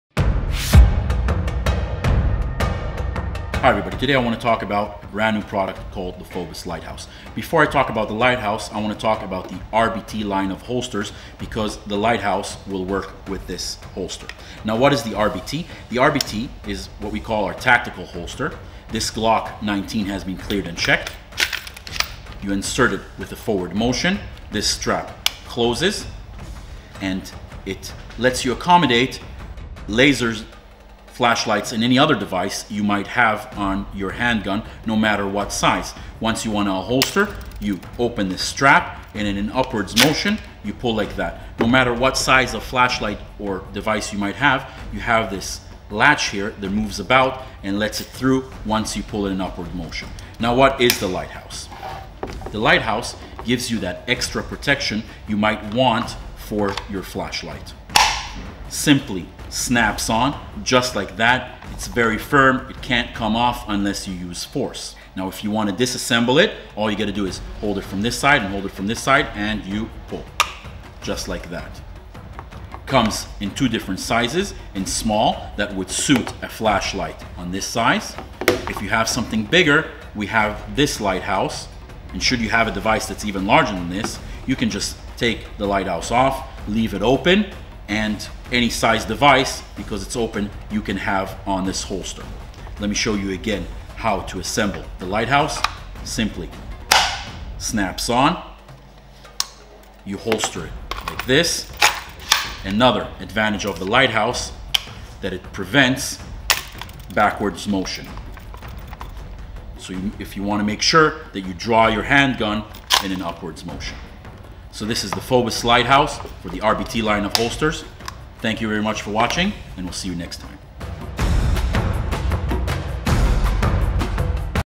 Fobus Ligh Bearing holster
Flashlight & Laser protection for Fobus RBT17G / RBT19G Glock 19/17 holsters.

Lasers & Flashlights fits:
INFORCE APLC
VIRIDIAN C5L
VIRIDIAN E-SERIES
LASERMAX SPARTAN
LASERMAX UNI
CRIMSON TRACE LIGHTGUARD
CRIMSON TRACE RAIL MASTER
SURFIRE XC1
STREAMLIGHT TLR
STREAMLIGHT TLR-1 & TLR-2
SUREFIRE X300 & X200
VIRIDAN X5L
INSIGHT M3X
AIMKON P10S